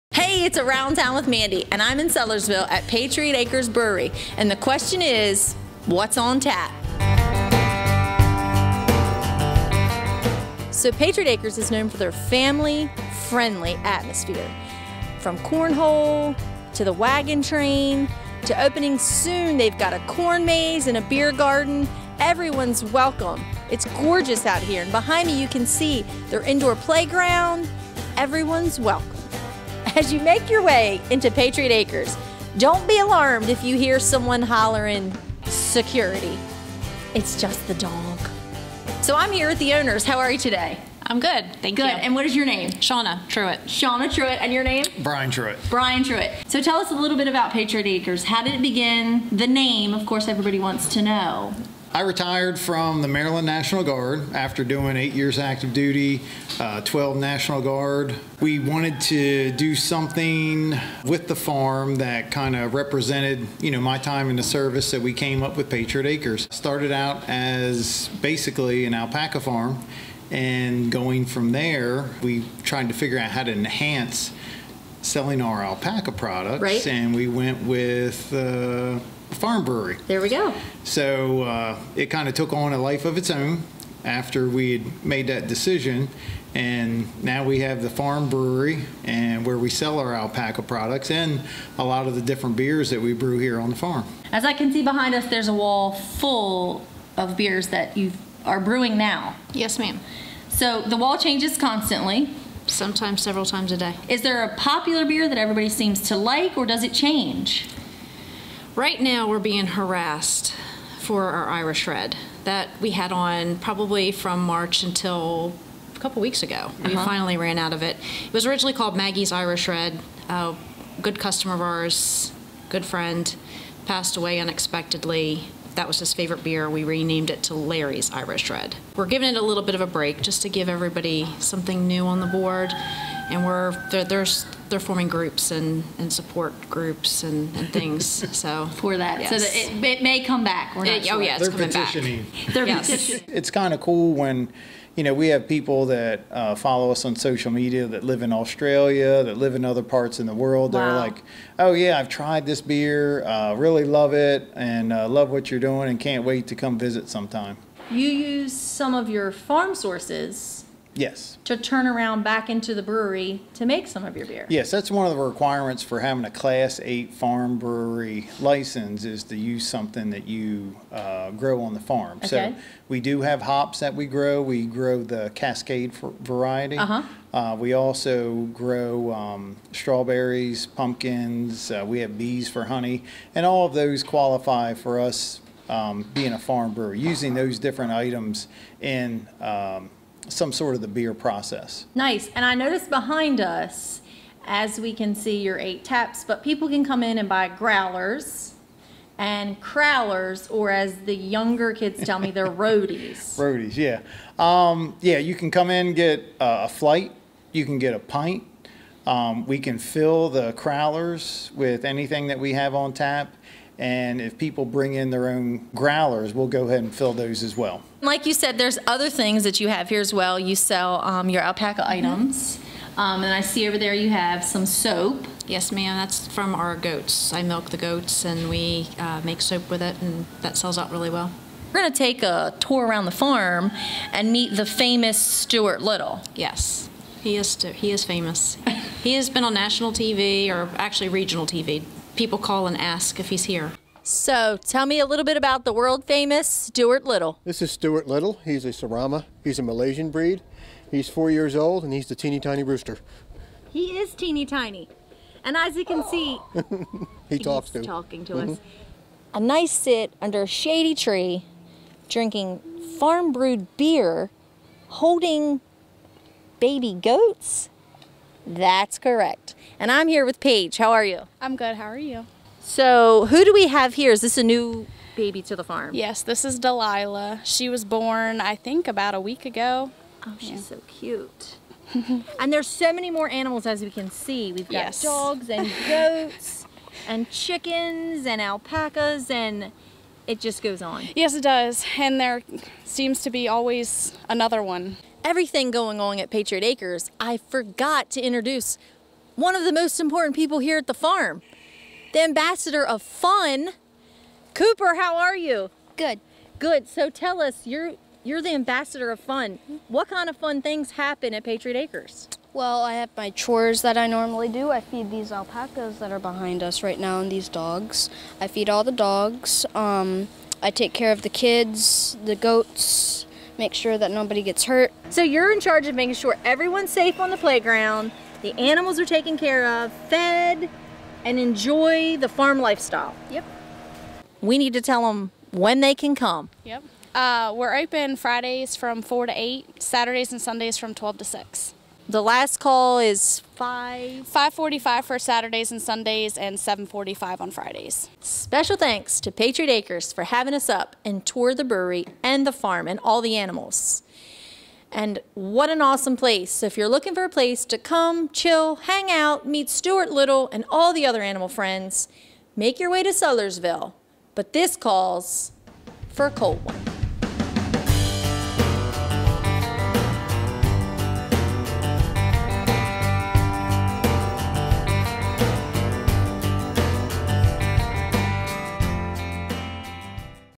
Around Town With Mandy Visits Patriot Acres Farm Brewery
Around Town With Mandy went up to Patriot Acres Farm Brewery in Sudlersville, Maryland. Patriot Acres goal is to provide their visitors good craft beer and an enjoyable atmosphere on a working farm. They use hops and other ingredients from the 100 acre farm that has been in the family for over a century. To learn more about them you can check them out on Facebook or you can visit their